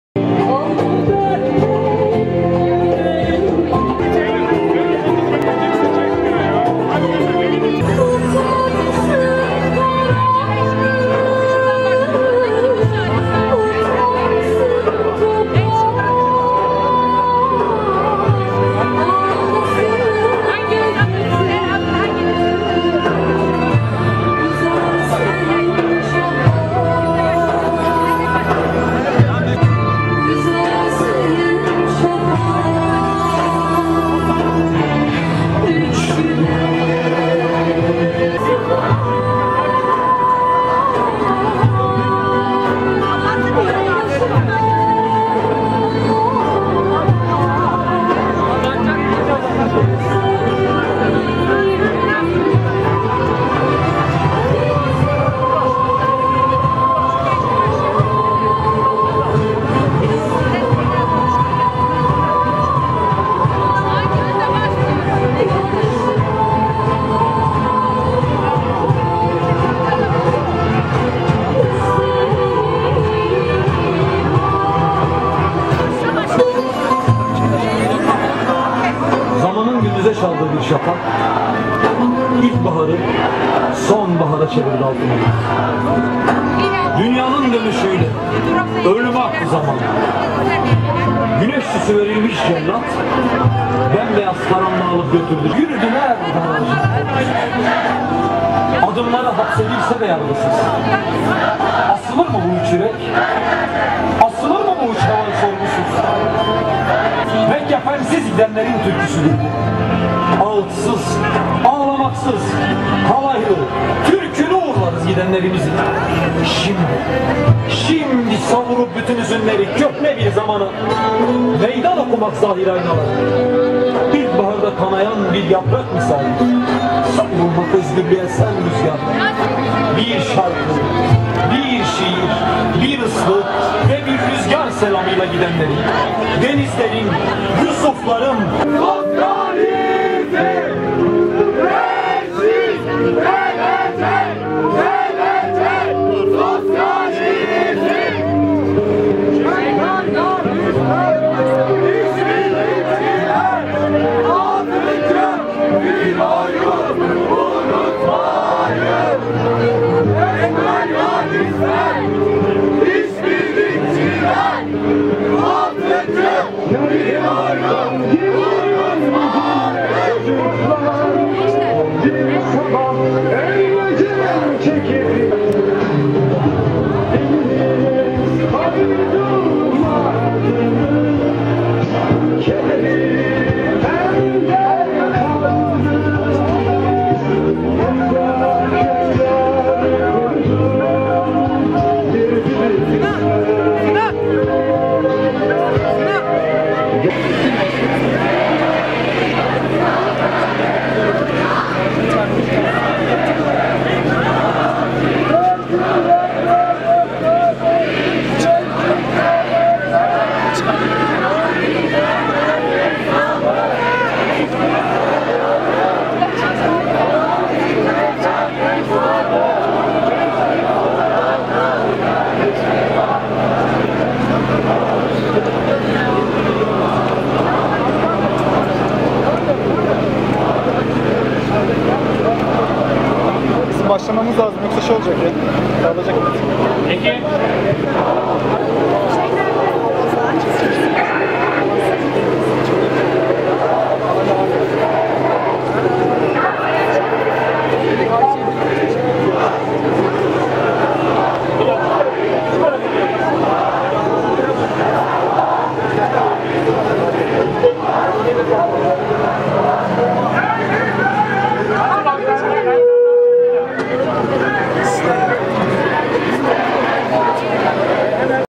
Karşıyaka Mezarlığı'nda 6 Mayıs anması
Deniz Gezmiş, Yusuf Aslan ve Hüseyin İnan'ın idamlarının yıldönümü anması